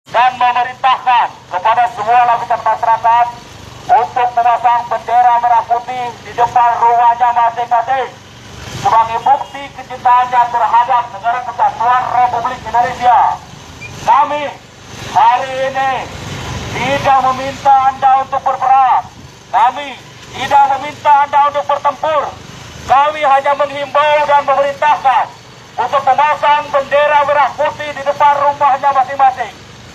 himbauan memasang bendera merah putih di rumah masing masing untuk memeriahkan hut RI
ajakan memasang bendera merah putih di rumah masing masing untuk memeriahkan hut RI